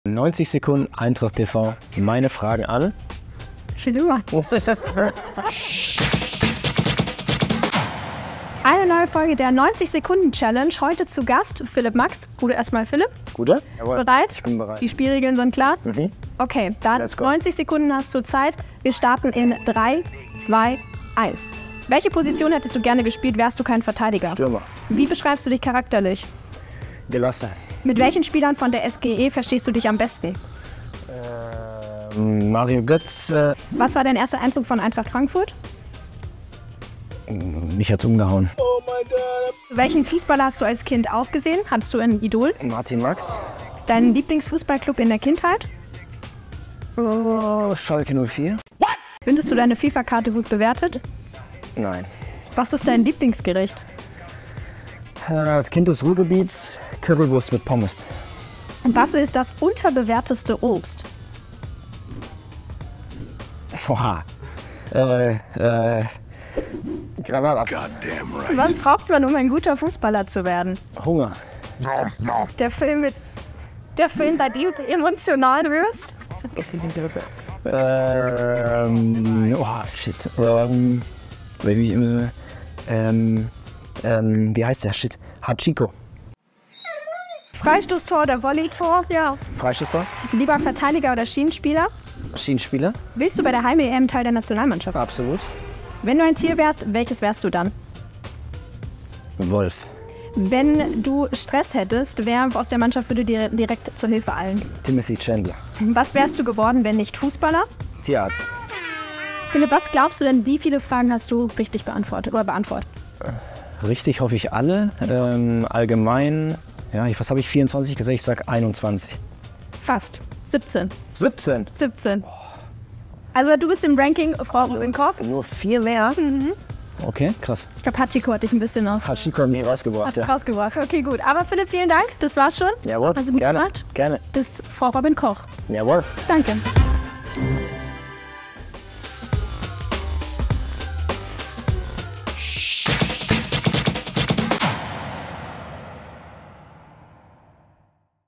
"Bei Hachiko werde ich emotional!" I 90 Sekunden Challenge mit Philipp Max
Die zweite Folge unserer 90 Sekunden Challenge! Wir haben euch gefragt, was ihr von Philipp Max schon immer mal wissen wolltet und haben ihn genau diese Fragen gestellt. Knackt er die Anzahl von Robin Koch? Präsentiert von Elotrans!